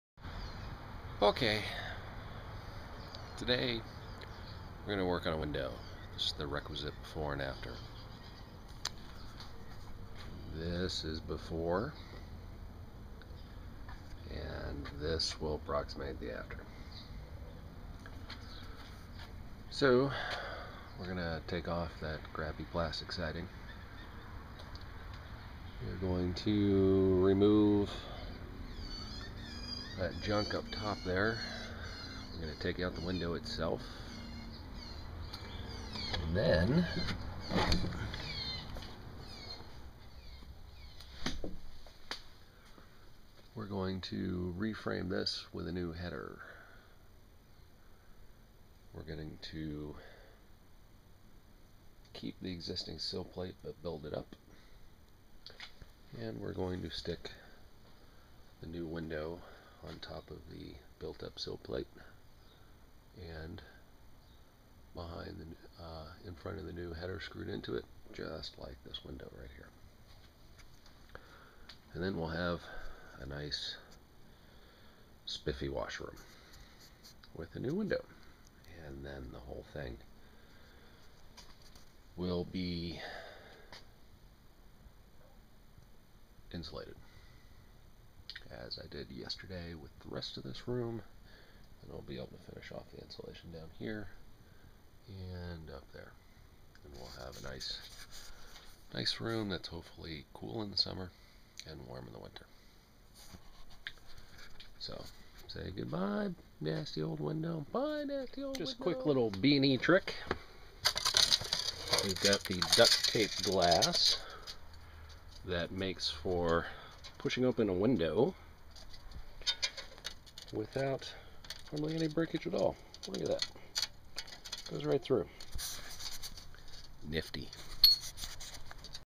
Removing window for remodel
Removing a window to reframe and replace it, part of a full Kitchen remodeling project